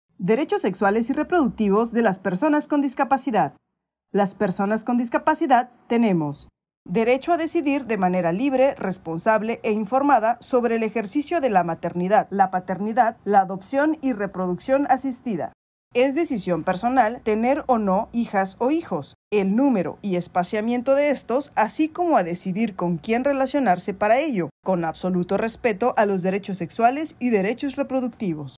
11. Derecho a decidir de manera libre, responsable e informada sobre el ejercicio de la maternidad
Derechos Sexuales y Reproductivos de las Personas con Discapacidad.
11. Derecho a decidir de manera libre, responsable e informada sobre el ejercicio de la maternidad, la paternidad, la adopción y la reproducción asistida.